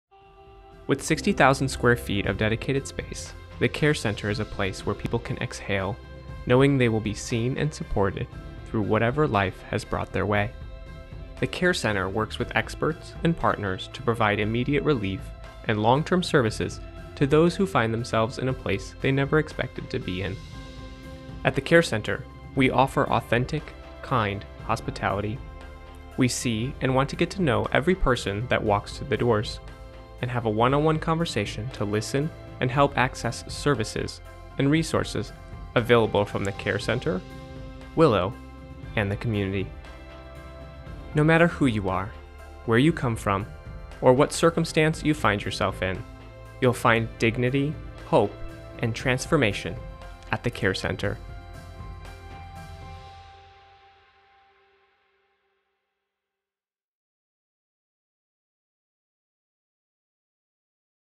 Willow Creek Care Center | About Video
The Willow Creek Care Center helps meet immediate and long-term needs that build toward a
sustainable future for individuals and families in the Chicagoland area.

Recorded for Willow Creek Care Center, a Willow Creek Community Church ministry in South Barrington, IL.